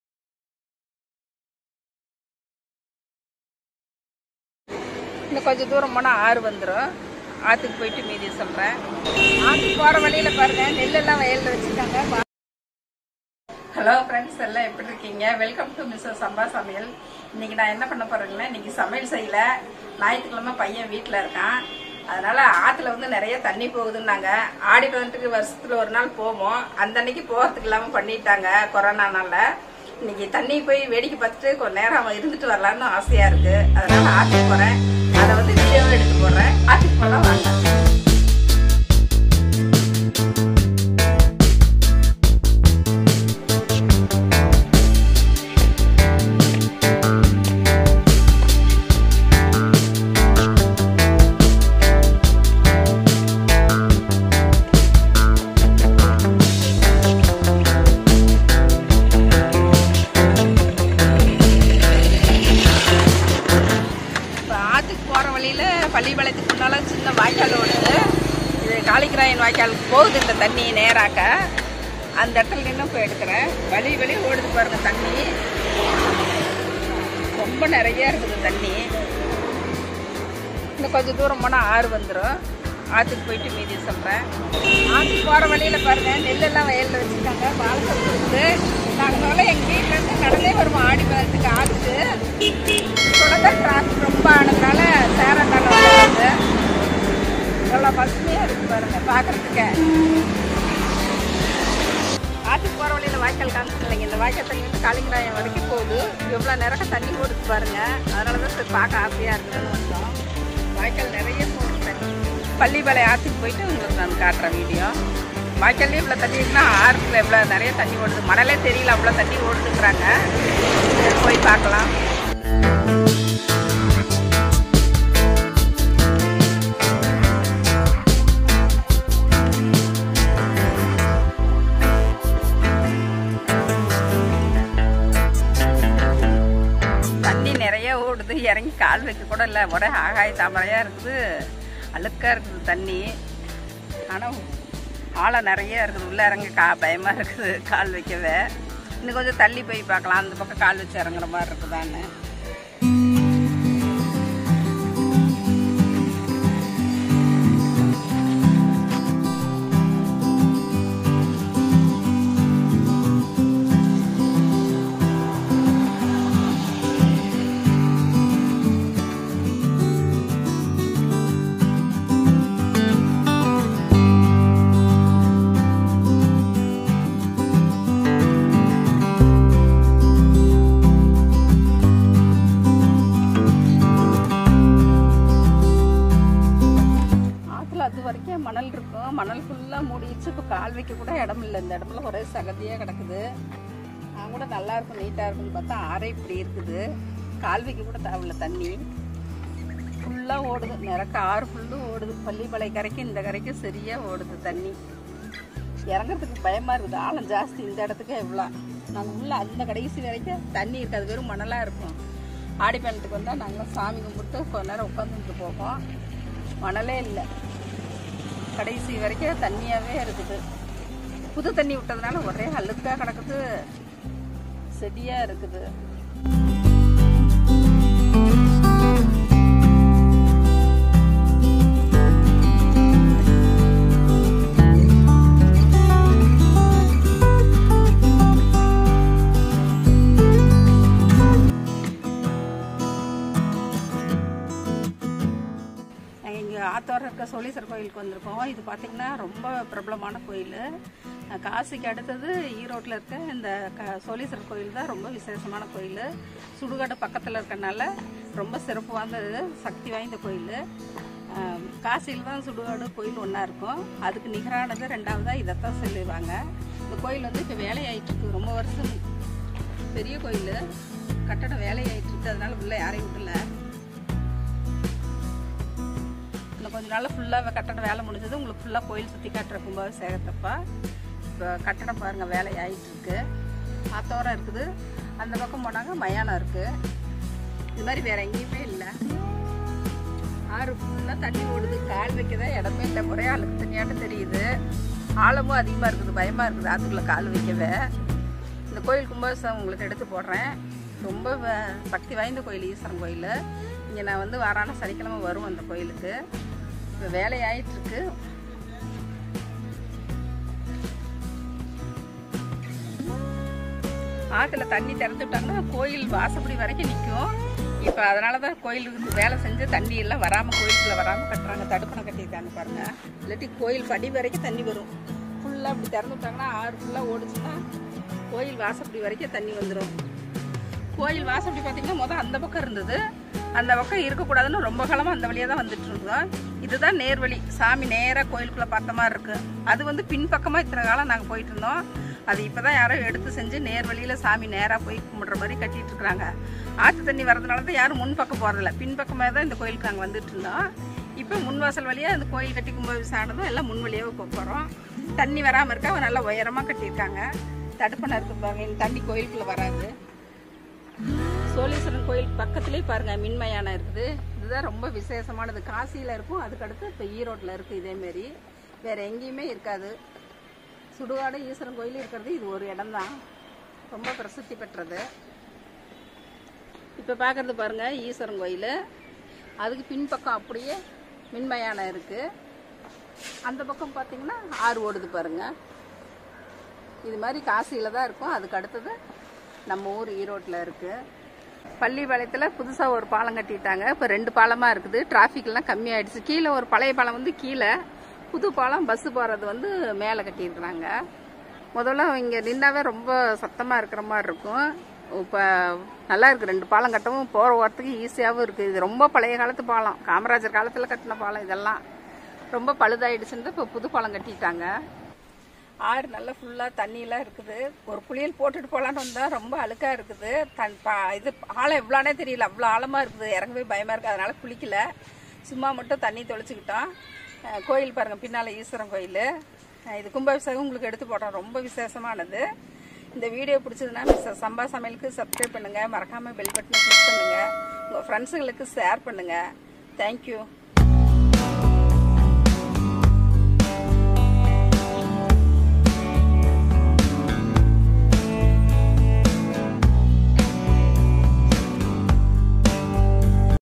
Travel Vlog to Pallipalayam River♥️ | சோழீஸ்வரர் திருக்கோயில் 🙏 | My Vlogs | Mrs Samba Samayal
Travel Vlog to Pallipalayam River♥️ | சோழீஸ்வரர் திருக்கோயில் 🙏 | My Vlog | Mrs Samba Samayal

 Hai foodies ,
I will show you something special  in this video.  Cooking is off 👎🏻Travel is on 👍🏻😍. Small trip to Pallipalaiyam river / சோழீஸ்வரர் கோயில் ஈரோடு (Erode).  I will  Explain you about this temple in short (இத்தல ஈசனான சோழீஸ்வரரும் இறைவி வேதநாயகியும் தம்மை வந்து தரிசிப்போரை அருள்மாரி பொழிந்து காத்து வருகின்றனர். பொய்கையாரால் நட்பு எனும் பூ மலர்ந்து சேரனின் உயிர் காத்த தலமாகிய பெருந்துறை வந்து ஈசனையும் அம்மையையும் தரிசித்து வளம் பெறலாம். ஈரோட்டிலிருந்து 20 கி.மீ. தொலைவில், பெருந்துறையில் கோட்டை பேருந்து நிலையத்தின் மிக அருகாமையிலேயே இந்த சோழீஸ்வரன் கோயில் அமைந்துள்ளது) next to kaasi this soleeswarar temple is situated near graveyard and river. Now reconstruction work is going on.  After few months  during Kumbhabhishekham (கும்பாபிஷேகம்) I will post further videos. Don't miss it if you are erodians  come visit during consecration ceremony  Take your relations to this temple those who are far away from erode and show it's speciality and get boons from powerful god Shivan .

    Thank u so much for watching 🙏🏻மிக்க நன்றி நேயர்களே